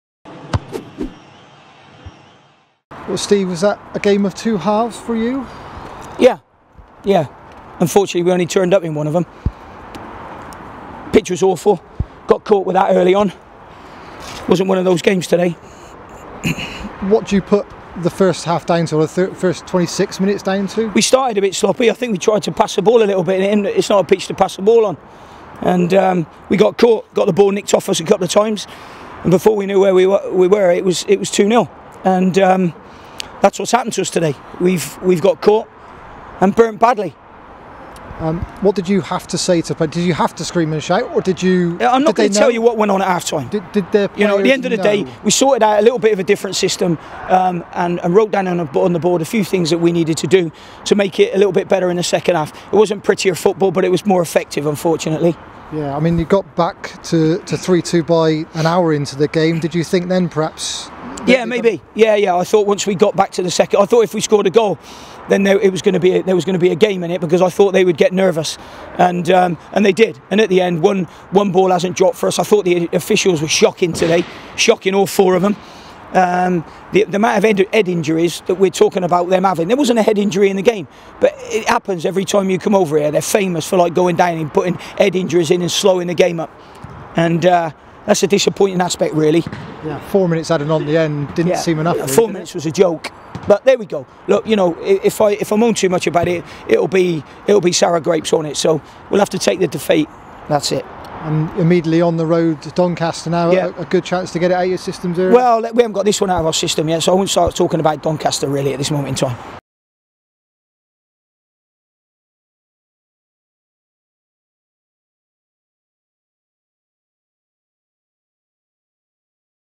Colchester United 3-2 Bristol City Post-Match Interviews
Manager Steve Cotterill aggrieved as City suffer their fifth defeat of the season away at Colchester.